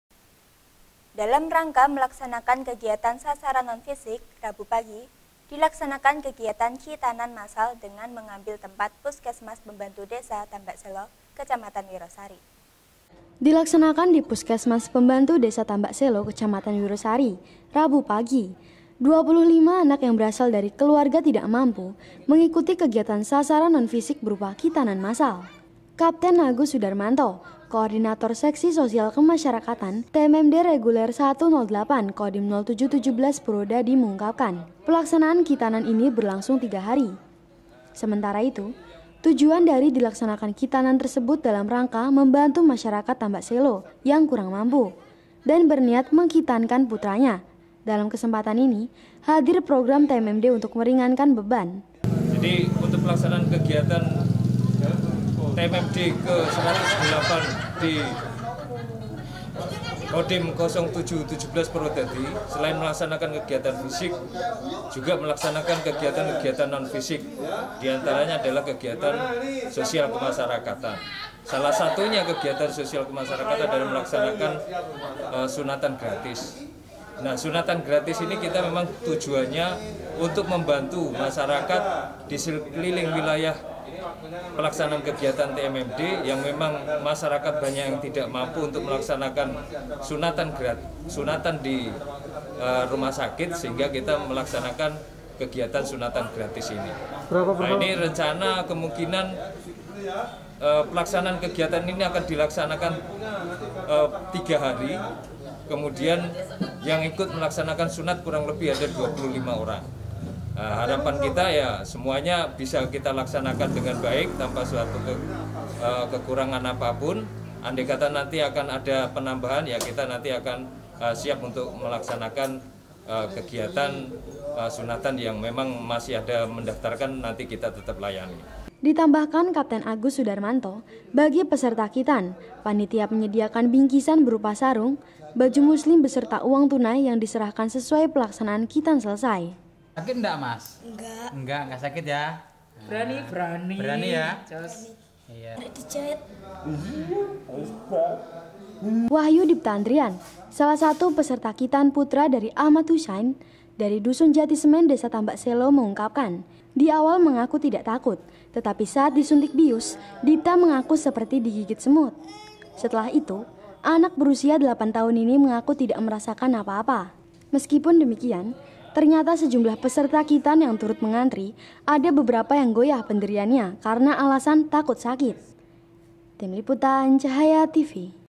Satgas TMMD Khitan Puluhan Anak Dari Keluarga Tidak Mampu
Dalam rangka melaksanakan kegiatan sasaran non fisik, Rabu pagi, dilaksanakan kegiatan khitanan massal dengan mengambil tempat  puskesmas pembantu Desa Tambakselo Kecamatan Wirosari.

Twitter :@cahayatvpati